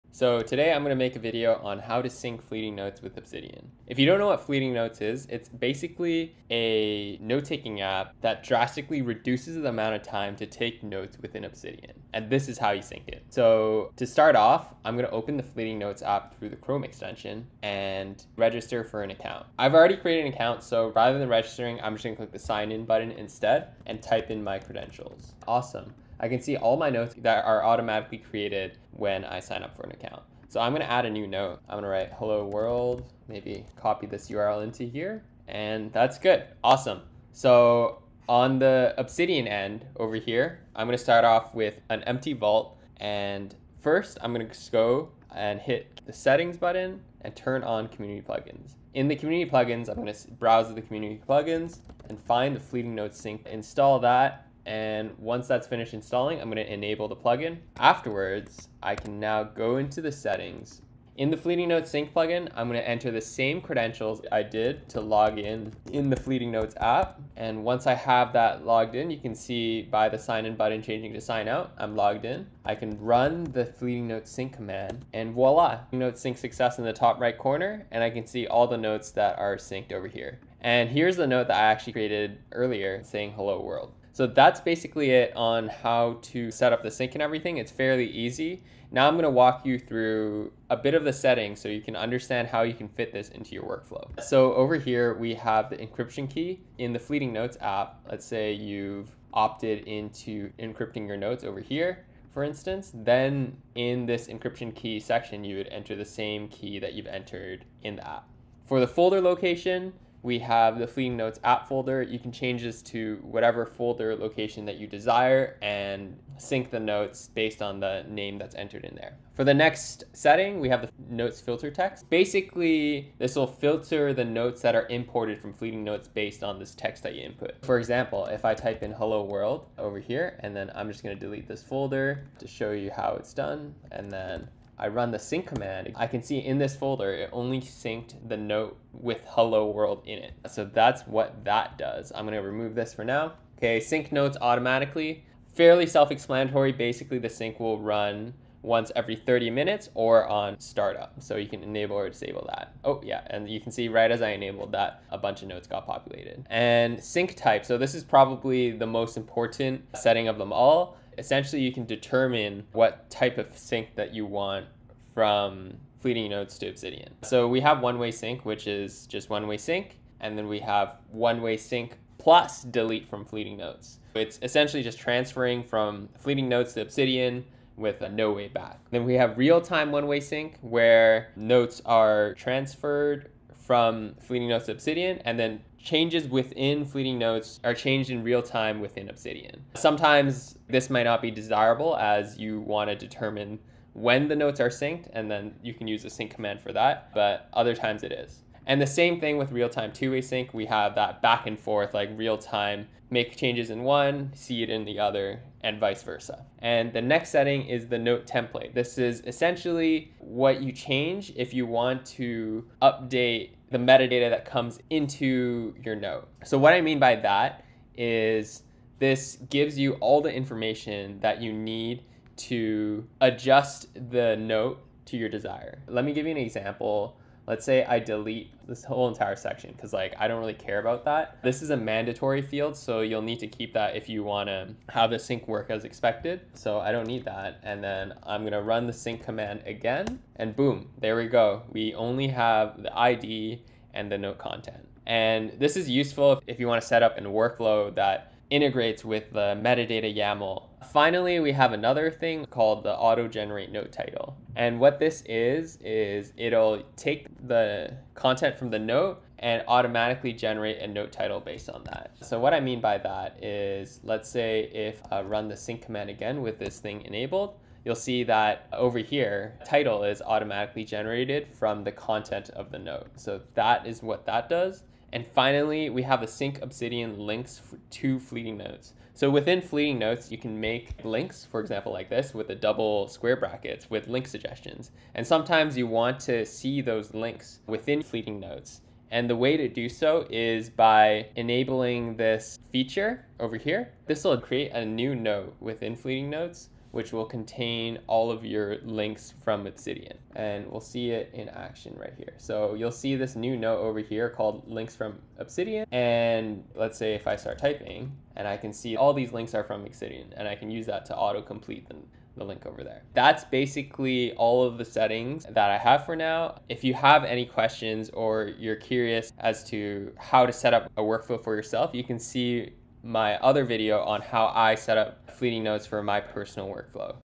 How to Sync Fleeting Notes with Obsidian
00:00 Intro
00:13 Register / Sign in For Account
00:38 Setup Obsidian plugin
1:29 Other Settings from the plugin